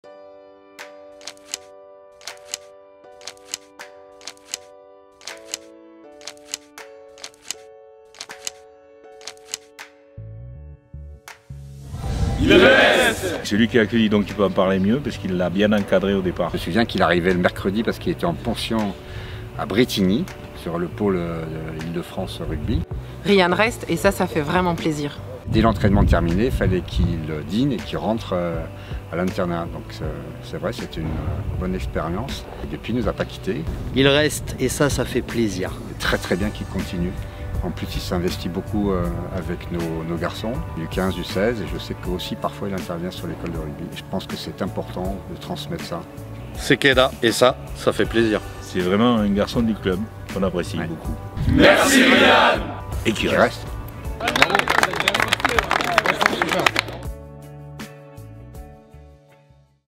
Ryan Chapuis prolonge jusqu'en 2024
Au-delà du troisième ligne, c'est un enfant du Club qui prolonge dans la capitale, tant avec l'équipe professionnelle qu'avec l'Association avec laquelle il est fortement engagé.